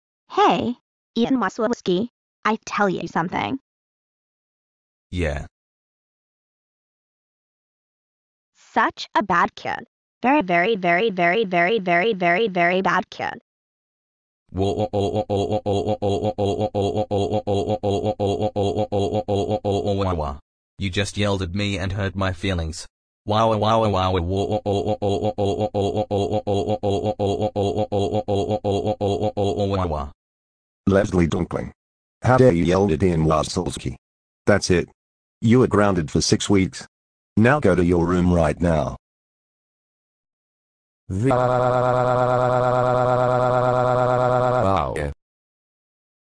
Leslie dunkling yelled at Ian wazselewski gets grounded
Cast
Leslie dunkling as princess
Ian wazselewski as Russell
Spot helperman as Alan